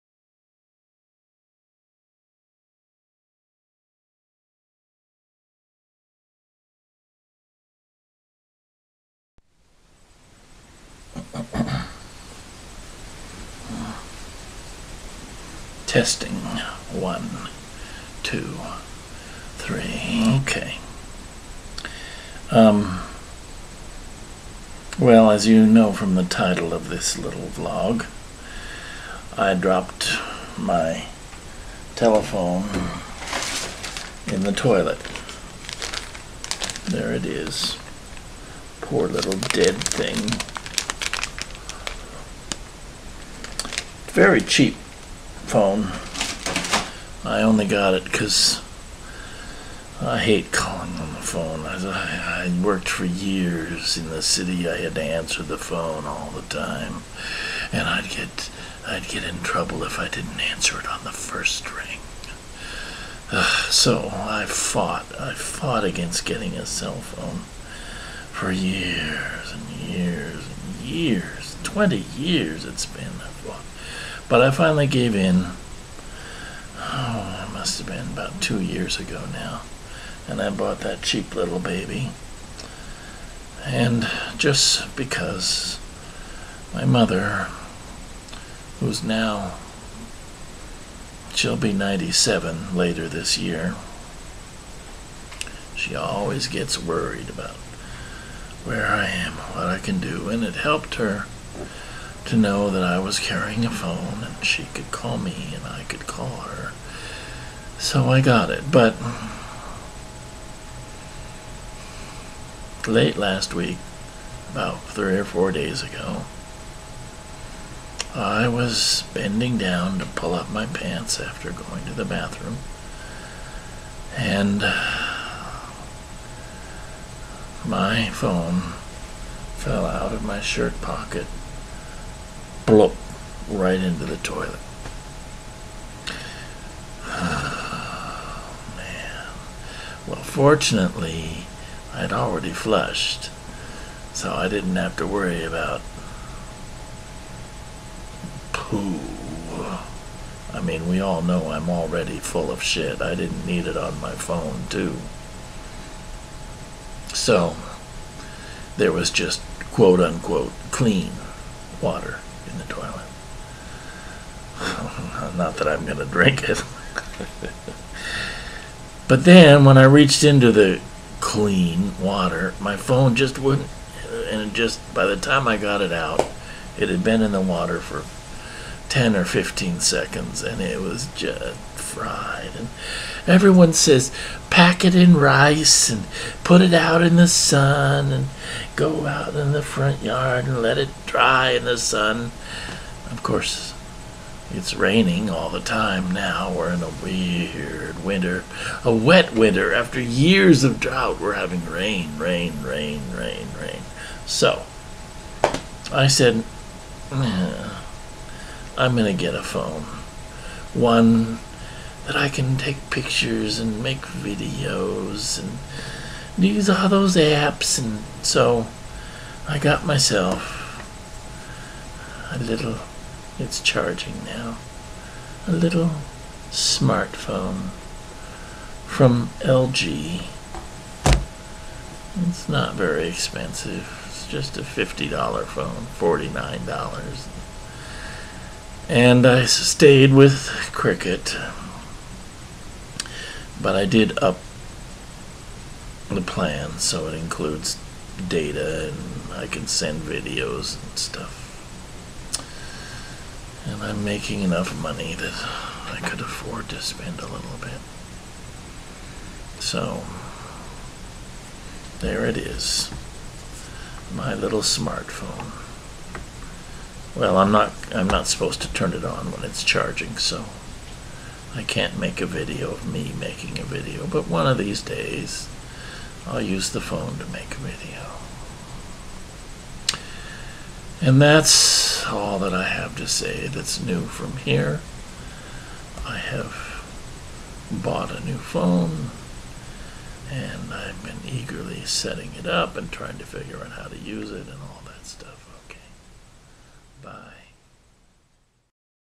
2017 01 18 my phone fell in the toilet a few days ago
and so I had to buy a new one oh yes I did